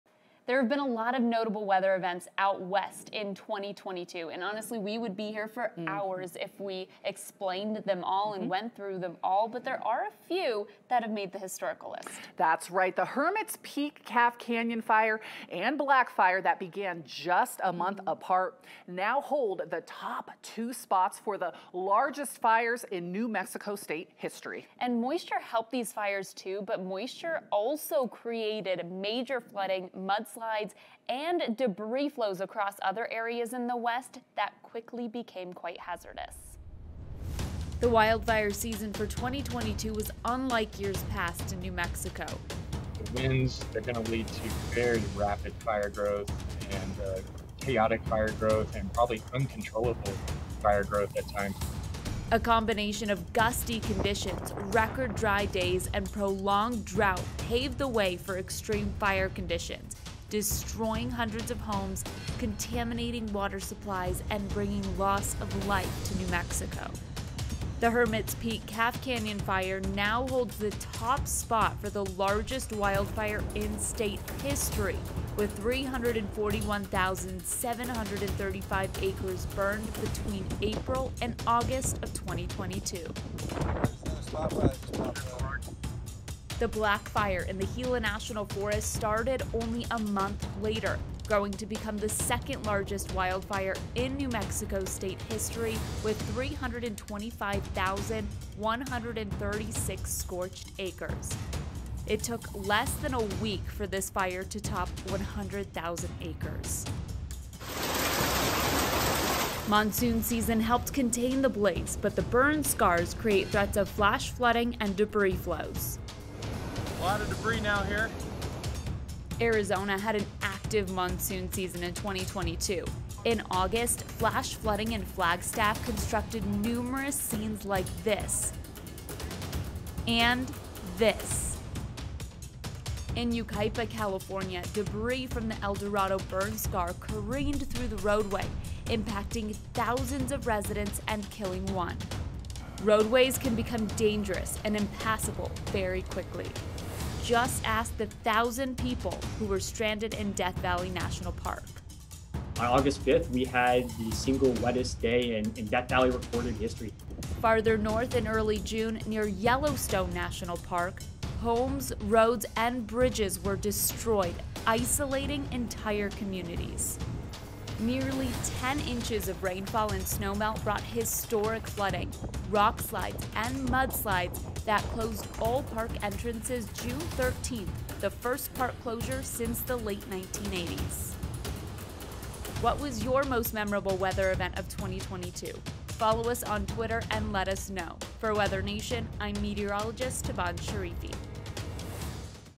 Notable Weather Events of 2022: West
(12/24/2022) – There were too many high impact weather events across the country to boil it down to just ten. Here's a look at some of the other weather headlines from the western U.S. this year.